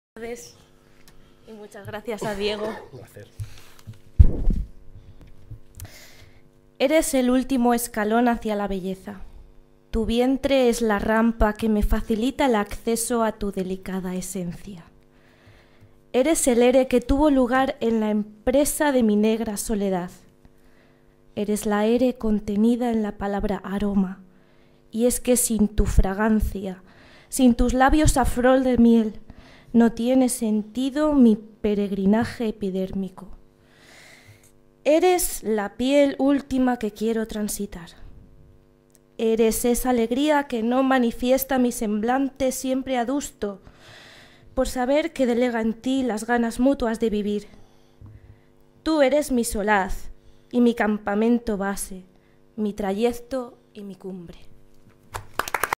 ''ESCALÓN HACIA LA BELLEZA'' (por SARA B VIÑAS)
“ESCALÓN HACIA LA BELLEZA”, poema de Diego Vadillo López recitado por SARA B VIÑAS en el recital “Léxico-semántica ludopatía”, que tuvo lugar en la Universidad Complutense de Madrid el pasado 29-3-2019.

Escalón hacia la belleza

Eres el último escalón hacia la belleza;
tu vientre es la rampa que me facilita el acceso a tu delicada esencia.
Eres el ere que tuvo lugar
en la empresa de mi negra soledad.
Eres la ere contenida en la palabra aroma,
y es que sin tu fragancia;
sin tus labios a flor de miel,
no tiene sentido mi peregrinaje epidérmico.
Eres la piel última que quiero transitar;
eres esa alegría
que no manifiesta mi semblante,
siempre adusto por saber que delega en ti
las ganas mutuas de vivir.
Tú eres mi solaz y mi campamento base;
mi trayecto y mi cumbre.